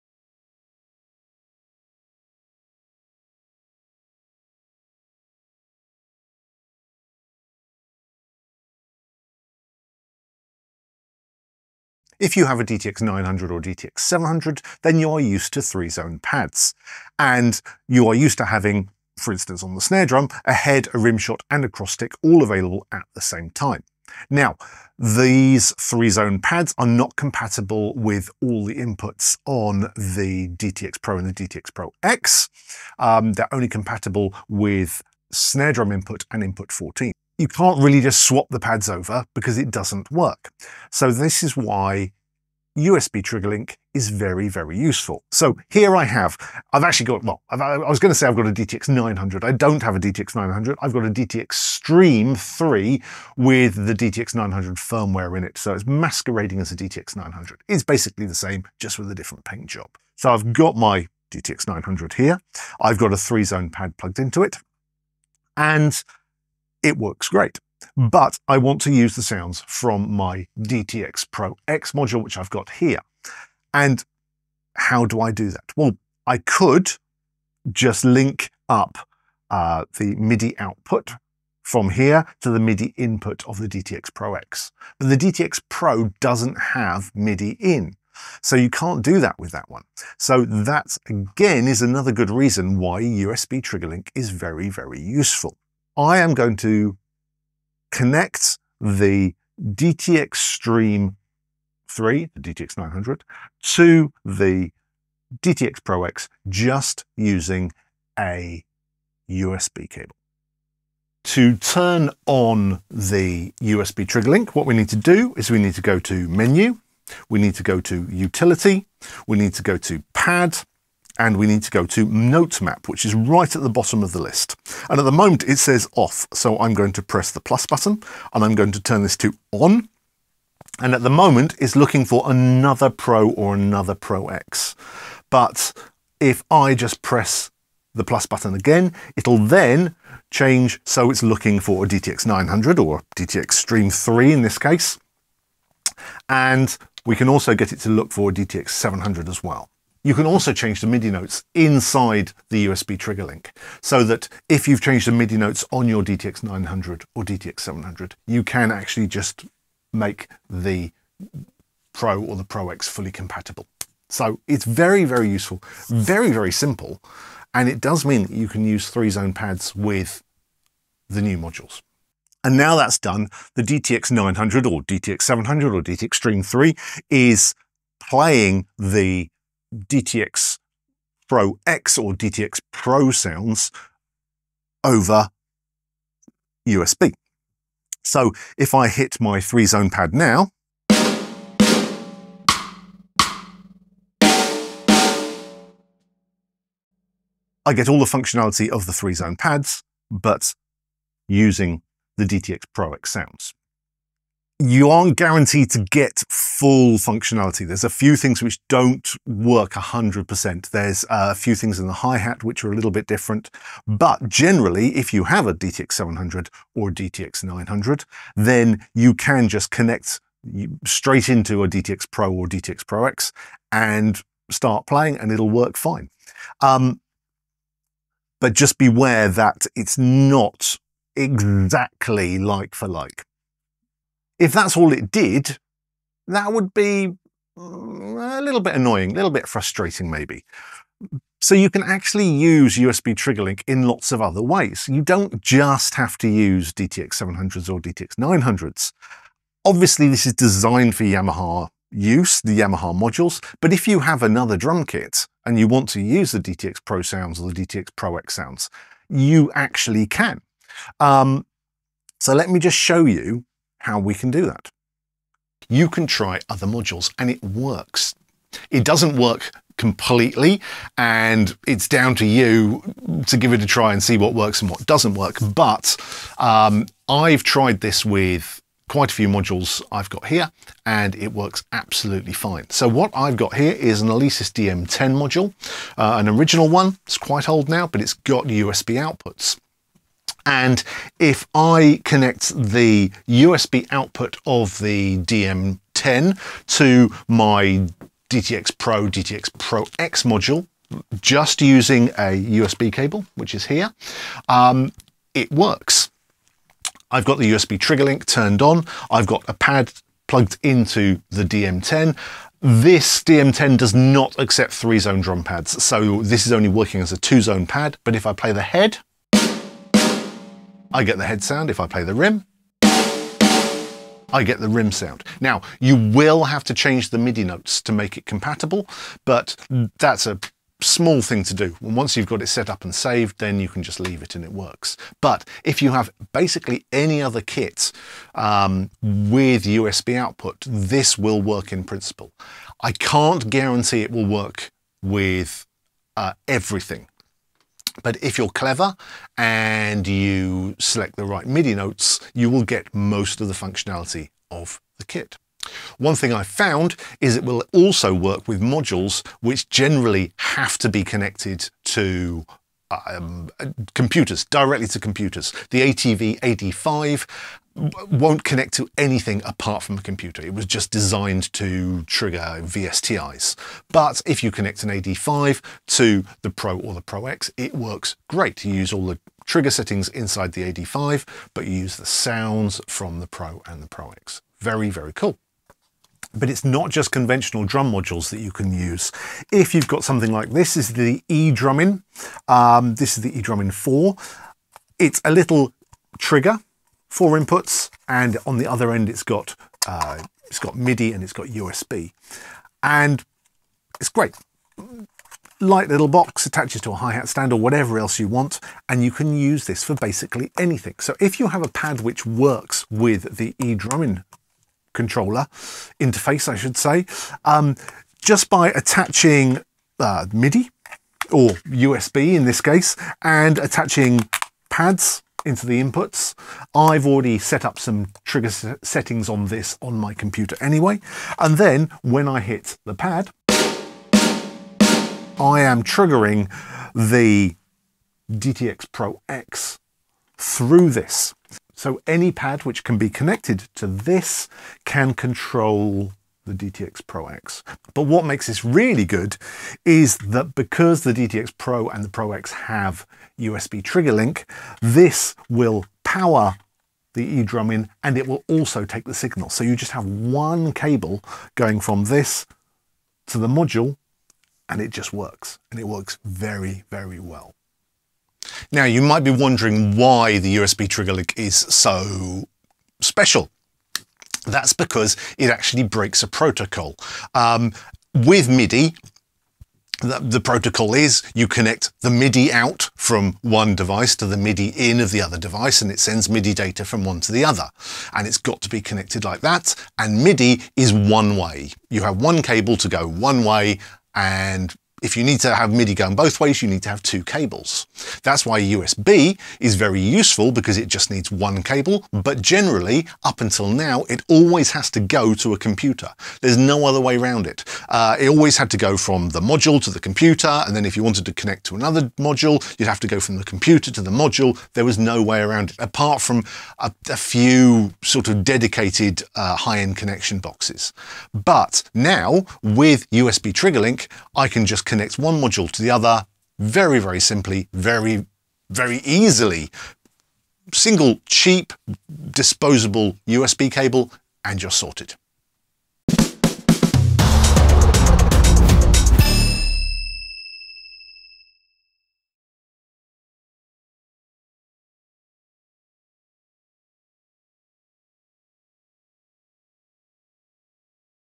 DTX V2 Firmware Update USB Trigger Link
A (not so ) short look at the USB Trigger Link feature on the new V2 DTX update. It's much more usable and flexible than the official use would suggest. If you want to use the DTXpro or the DTXproX modules as sound expanders for your existing kit, this is the video for you.